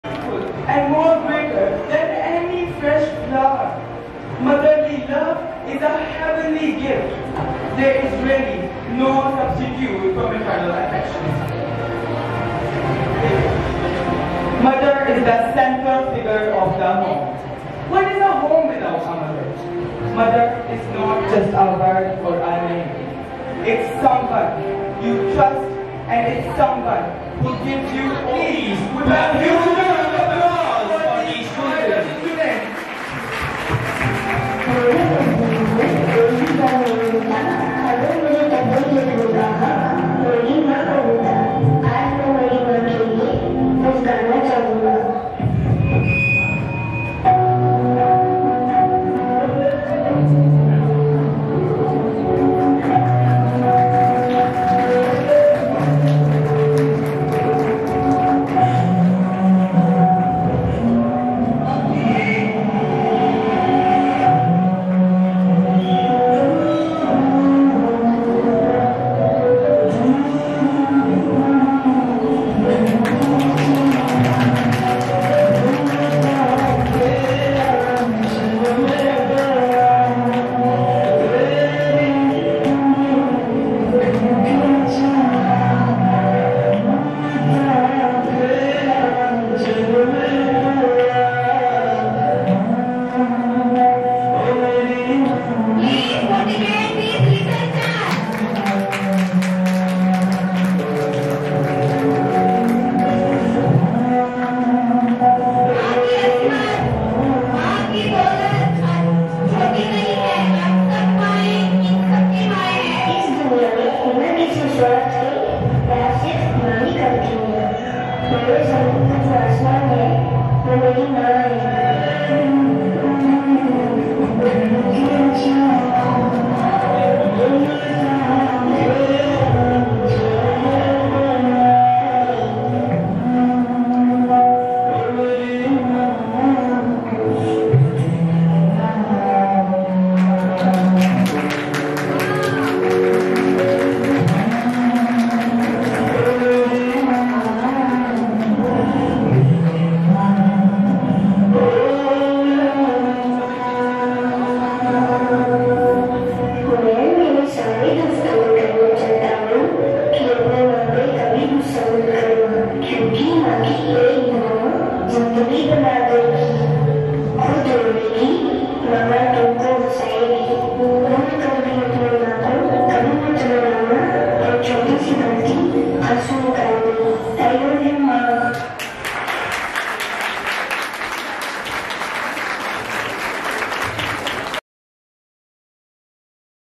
Tableau to Tribute to mother Tableau by Dar- e- Arqam school lasani town campus students
Dar- e -Arqam school lasani town campus students